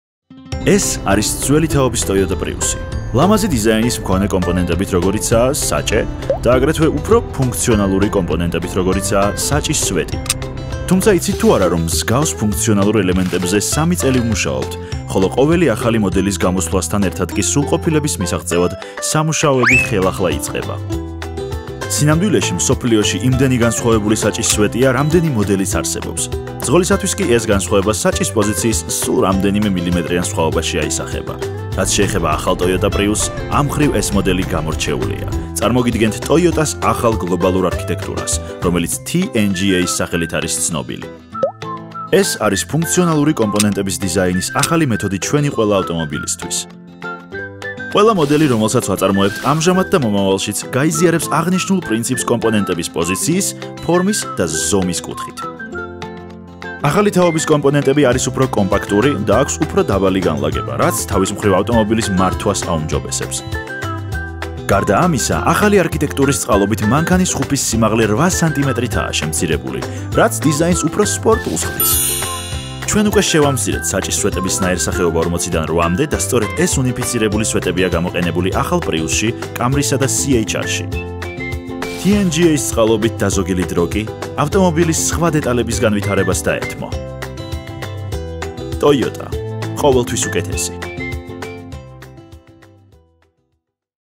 TNGA - ტოიოტას ახალი გლობალური არქიტექტურა
ისევე, როგორც ადამიანის ორგანიზმს, ასევე ავტომობილსაც ესაჭიროება ფუნდამენტალურად ძლიერი ბირთვი. სწორედ ეს არის ის, რასაც TNGA უზრუნველყოფს: ტოტალური ოპტიმიზაციის კონცეფცია, რაც გულისხმობს როგორც ცალკეული ნაწილების, ასევე ავტომობილის კომპლექსურ განვითარებას. TNGA არის დიზაინის, ინჟინერიის და ავტომობილების დაკომპლექტების სრულიად ახალი სტრატეგია.

შედეგად, ჩვენ ვიღებთ უფრო მსუბუქ და მაღალი დინამიური მახასიათებლების მქონე პლატფორმას, რაც მანქანის მართვას კიდევ უფრო სასიამოვნოს ხდის.